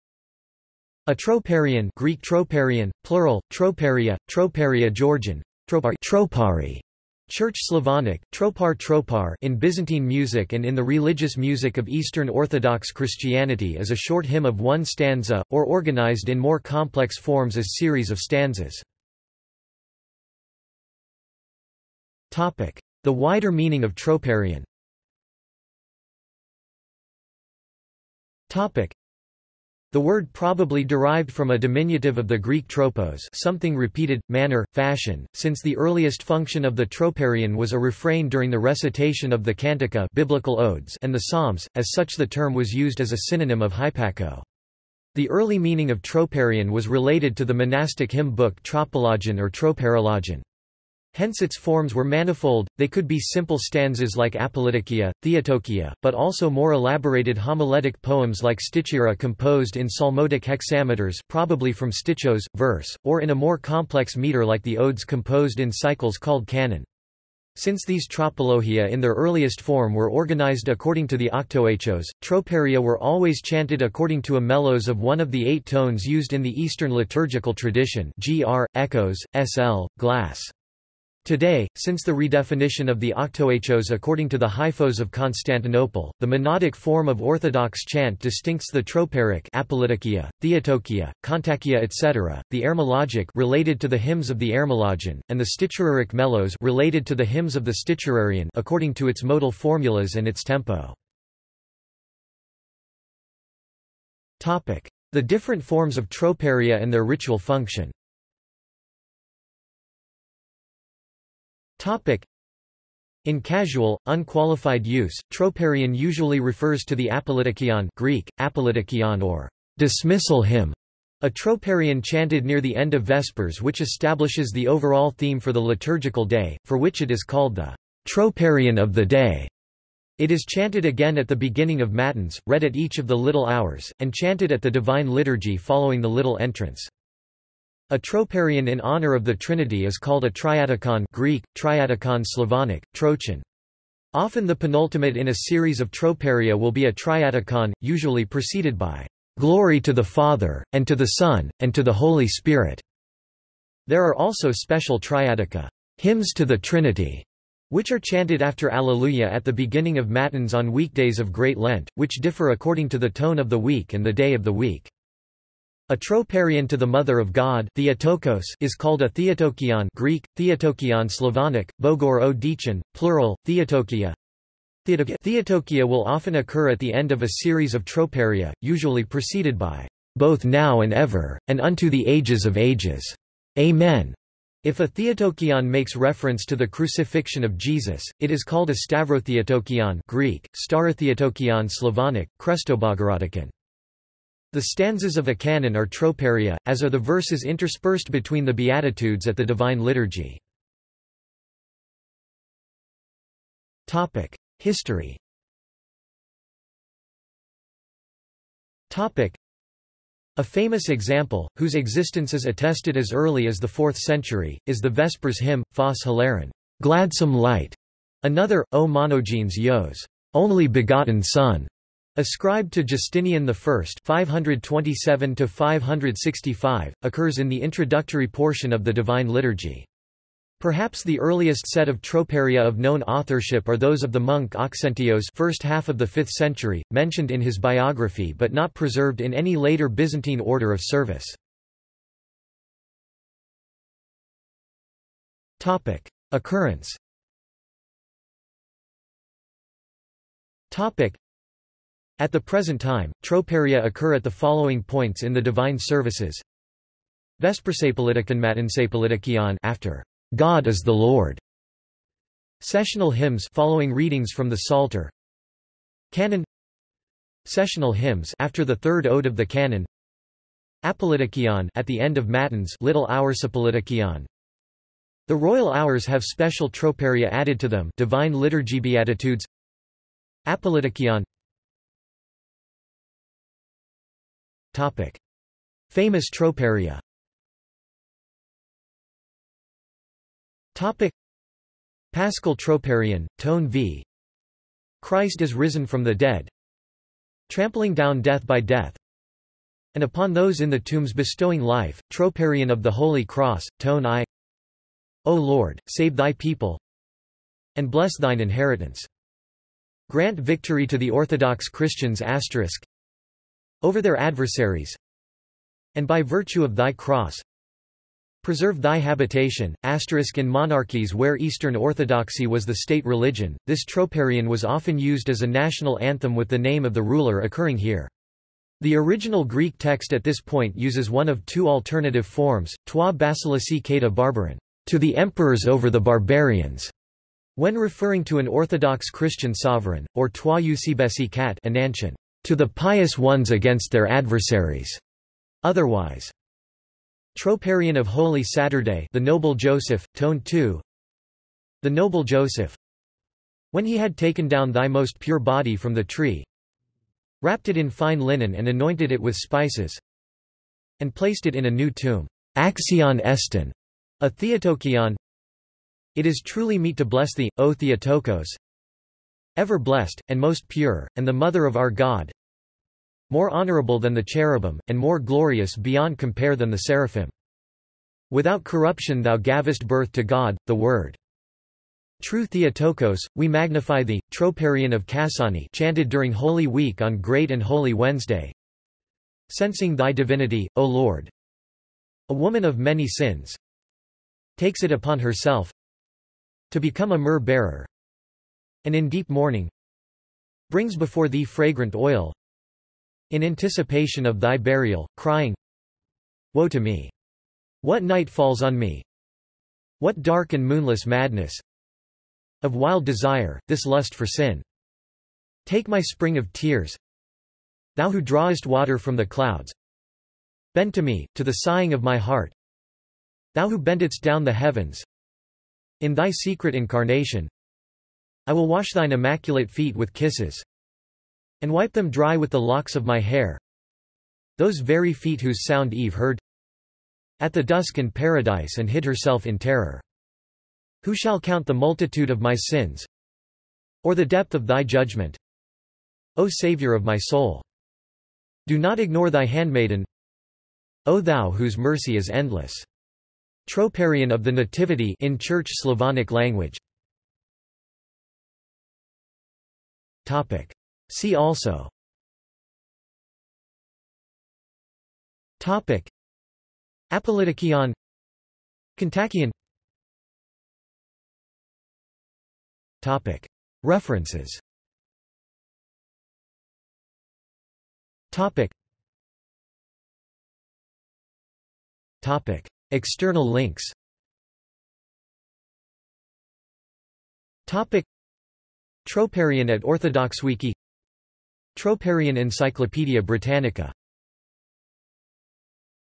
Troparion | Wikipedia audio article
This is an audio version of the Wikipedia Article:
Troparion

SUMMARY
A troparion (Greek τροπάριον, plural: troparia, τροπάρια; Georgian: ტროპარი, "tropari" Church Slavonic: тропа́рь, tropar) in Byzantine music and in the religious music of Eastern Orthodox Christianity is a short hymn of one stanza, or organised in more complex forms as series of stanzas.